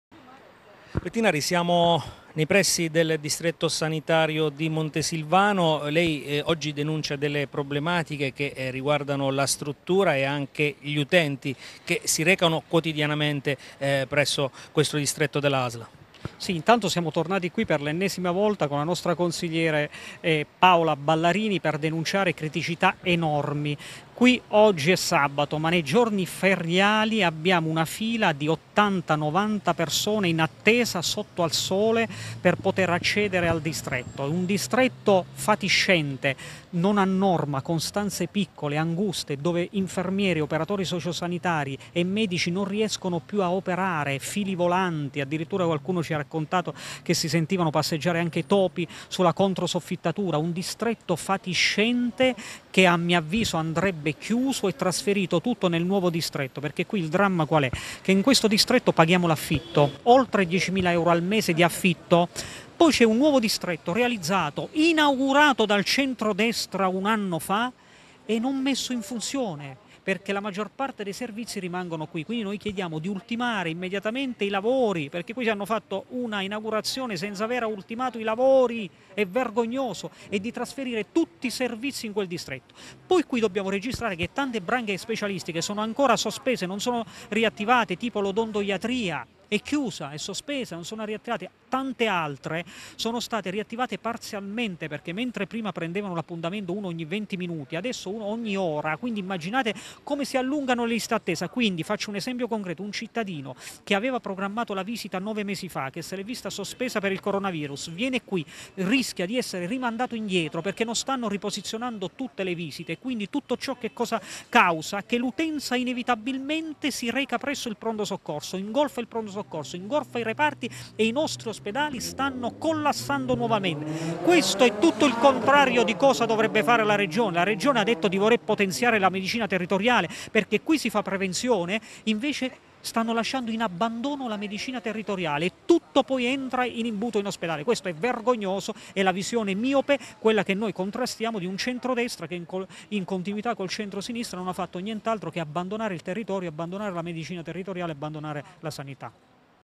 Pettinari denuncia problemi al Distretto ASL di Montesilvano
Il consigliere regionale del M5S Domenico Pettinari denuncia una situazione insostenibile al Distretto Sanitario di Base dell ASL a Montesilvano